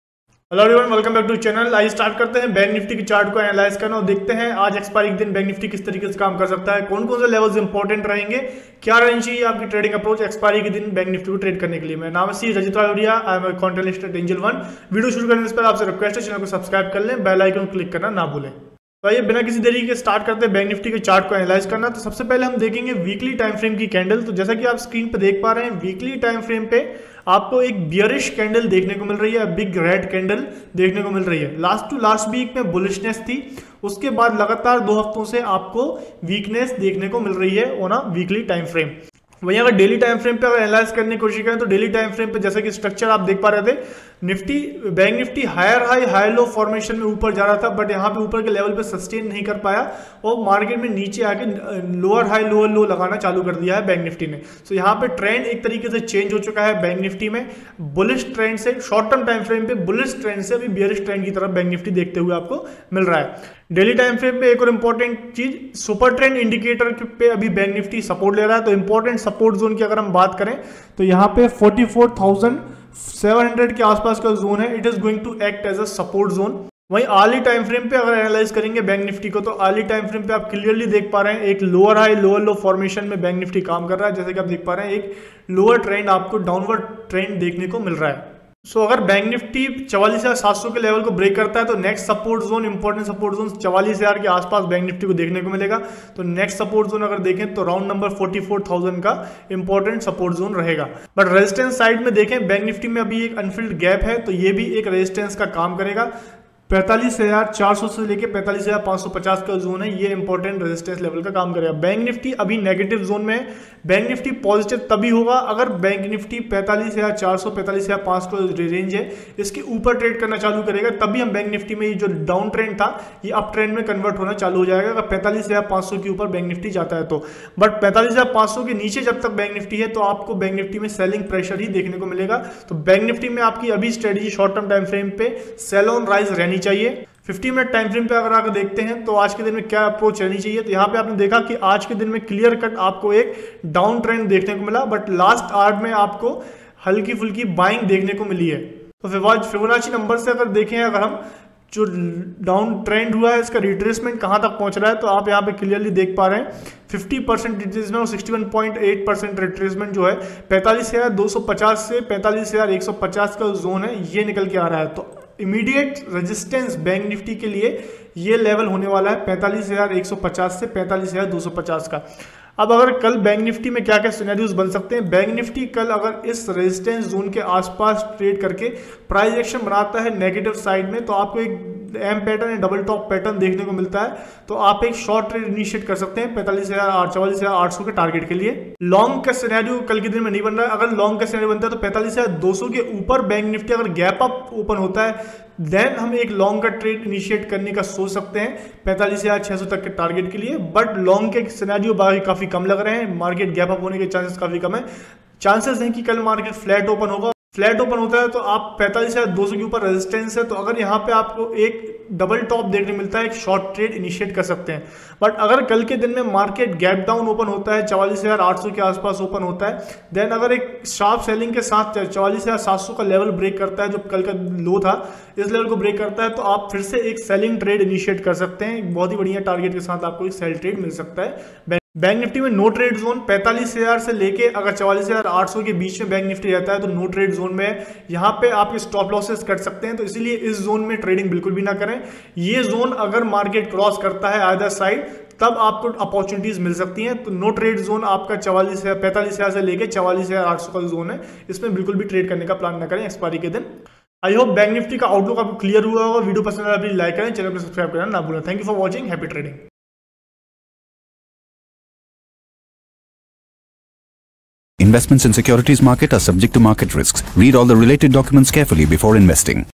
BankNifty Expiry Day Strategy | BankNifty Prediction for 3rd Aug
Looking for a winning strategy for BankNifty Expiry Day on 3rd Aug? Look no further!

0:00 - Introduction
0:18 - BankNifty in Weekly Chart
0:51 - BankNifty in Daily Chart
1:24 - BankNifty in Hourly Chart
2:04 - BankNifty in 15 mins Chart
2:56 - BankNifty through Finvasia Retracement
3:22 - Support & Resistance
3:43 - Prediction on BankNifty

Latest Videos to Watch:-

Give us Thumbs Up 👍, Comment 💬 & Share 🔗 this video !!